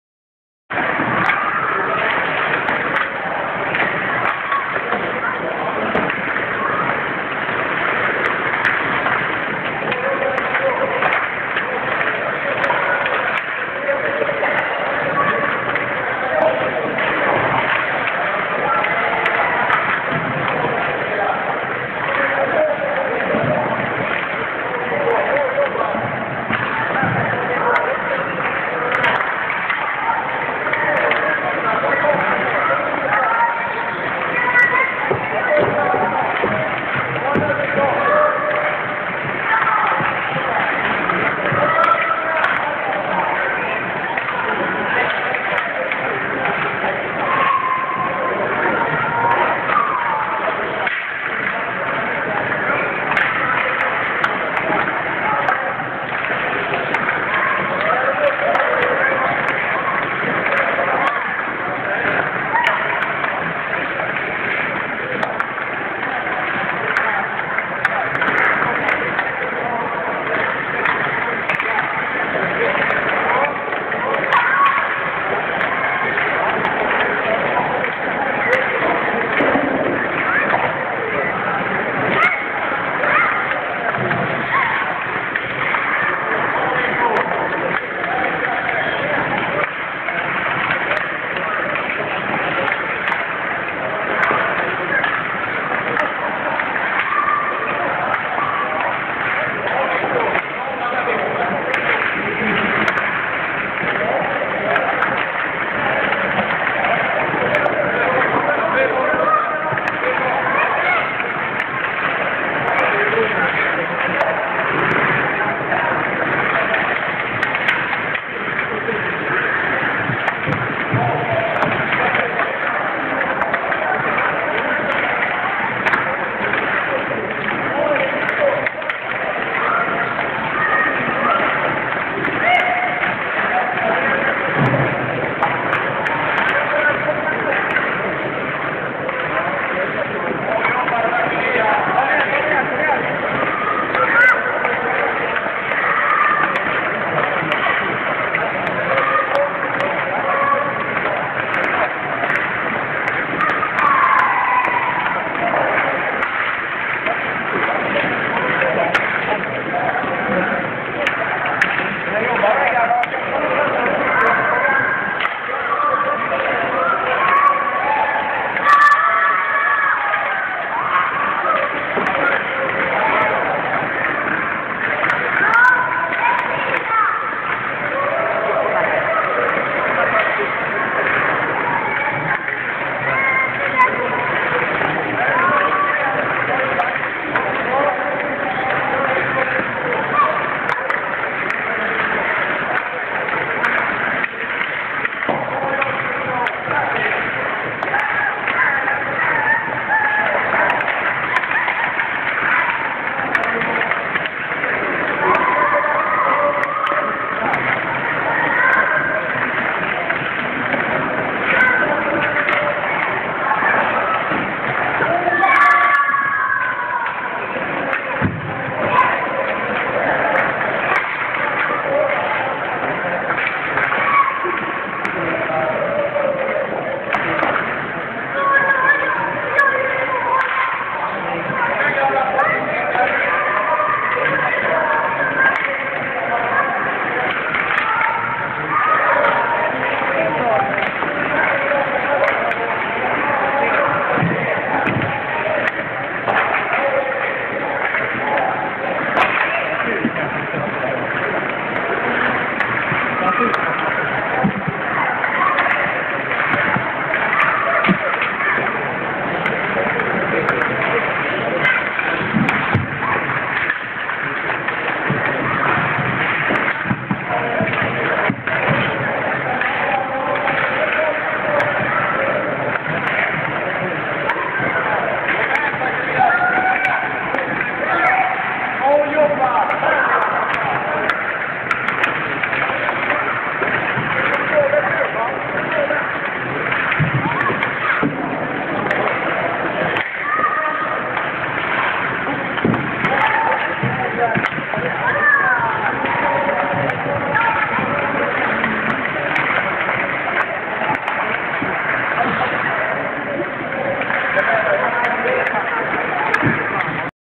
KHK Hockeyskola, lördag 19 Februari 2011
Ett kort klipp på KHK Hockeyskola inspelat lördag 19 Februari 2011.